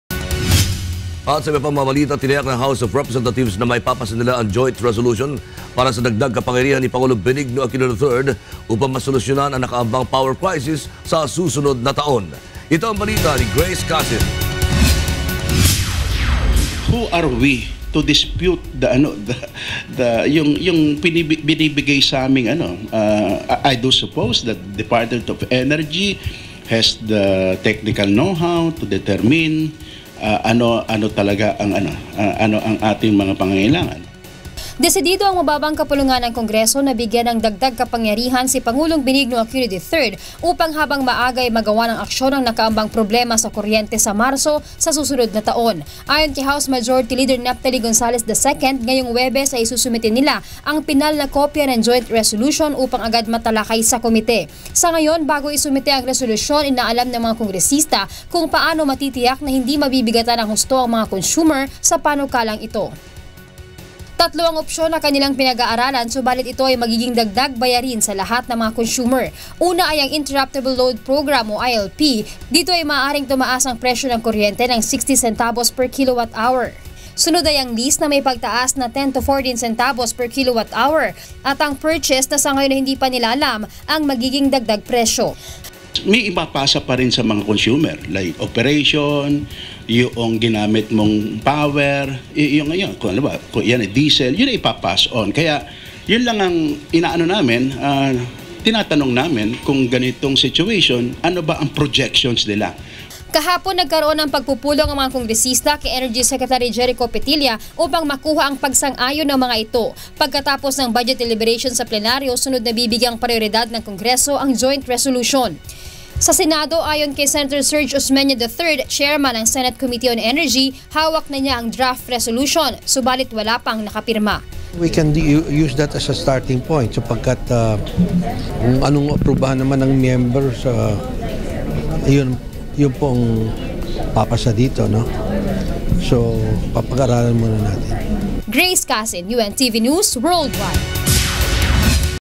Panukalang additional power kay Pres. Aquino, tiniyak ng HOR na maipapasa sa tamang panahon
Tiniyak ng House of Representatives na maipapasa nila ang joint resolution para sa dagdag kapangyarihan ni Pangulong Benigno Aquino III upang masolusyunan ang nakaambang power crisis sa susunod na taon.

Ito ang balita ni Grace Casin.